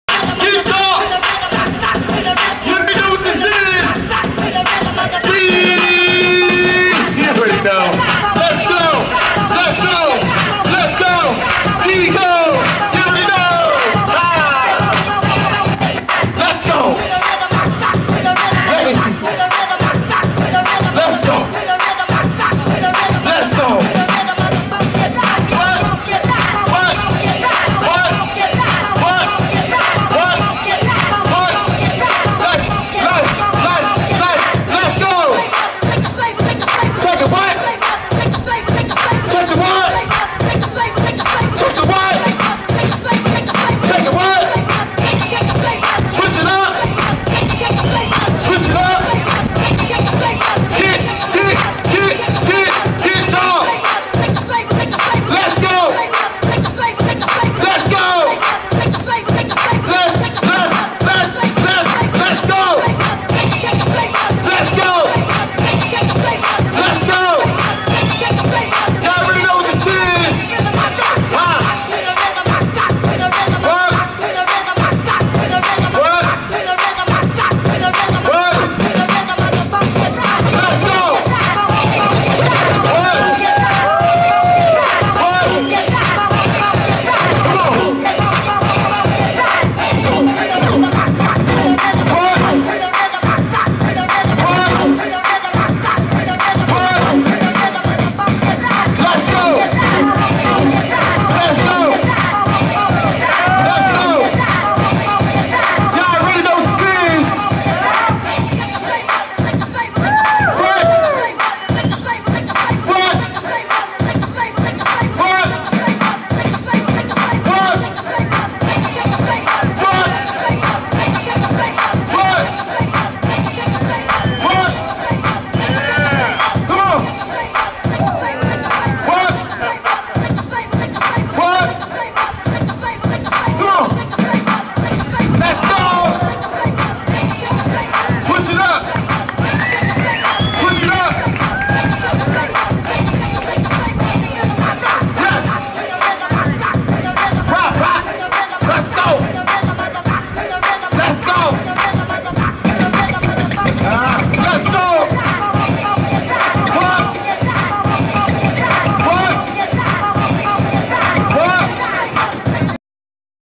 5Pointz breakdancing 3
Opening Day 2011 5Pointz Queens.. breakdancers having fun..